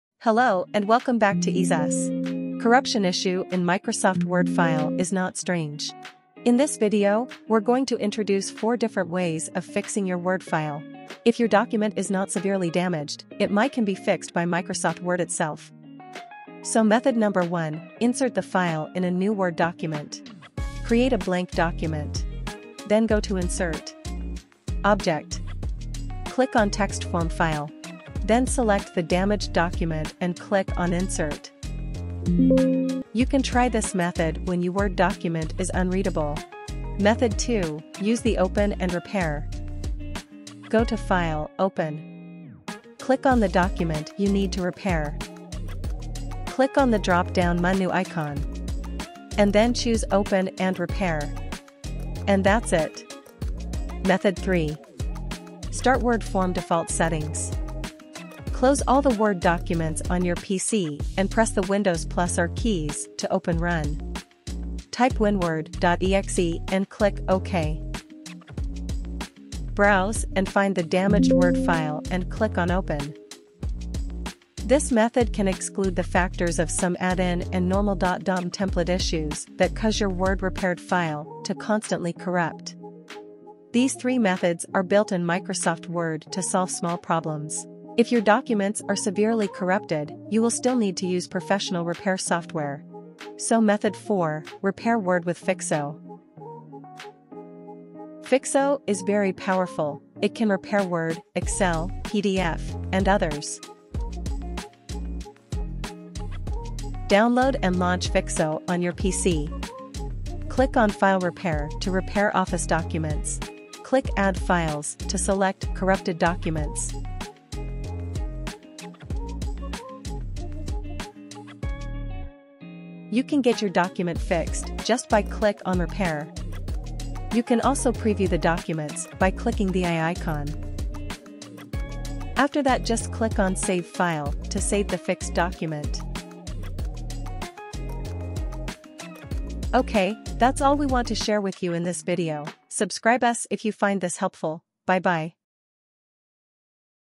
How to Repair Corrupted Word Document in 4 Ways [NEW]
This video tutorial shows how to repair corrupted Word documents with EaseUS Fixo Document Repair.

Microsoft Word file corruption is not rare. In this video, we will share 4 methods to help you repair corrupted/damaged/broken Word documents by using Microsoft Word itself and EaseUS Fixo Document Repair. You can also try these methods if you meet PPT/Excel corruption. This all-in-one repair program can help you repair corrupted videos, photos, and other documents.

★★★TIME CODE★★★

00:00 Introduction
00:14 Method 1. Insert to a new Word document
00:38 Method 2. Use open and repair
00:55 Method 3. Default settings
01:30 Method 4. EaseUS Fixo Document Repair

What to do when you encounter Word document corruption?

Method 1. Insert to a new Word document
Step 1. In Word, select "File" and then choose "New".
Step 2. Select "Blank document", and then select "Create".
Step 3. Next, choose "File" and then select "Open".
Step 4. Select the damaged paper, and then select "Open".
Step 5. Copy the contents of the document, and then paste the contents into the new Microsoft Word.

Method 2. Use open and repair
Step 1. In Word, select "File" on the "Ribbon", and then select "Open".
Step 2. In the "Open" dialog box, click once to highlight your Word document.
Step 3. Select the arrow on the "Open" button, and then choose "Open" and "Repair".

Method 3. Default settings
Step 1. Close all the Word documents on your PC.
Step 2. Press the Windows+R keys to open "Run".
Step 3. Type winword.exe /a and click "OK".
Step 4. Once Word opens up, go to "File" and click on "Open".
Step 5. Browse to the damaged Word file and click on "Open".

Method 4. EaseUS Fixo Document Repair
Step 1. Download and launch Fixo on your PC or laptop. Choose "File Repair" to repair corrupted Office documents, including Word, Excel, PDF, and others. Click "Add Files" to select corrupted documents.
Step 2. To repair multiple files at once, click the "Repair All" button. If you want to repair a single document, move your pointer to the target file and choose "Repair". You can also preview the documents by clicking the eye icon.
Step 3. Click "Save" to save a selected document. Select "Save All" to save all the repaired files. Choose "View Repaired" to locate the repaired folder.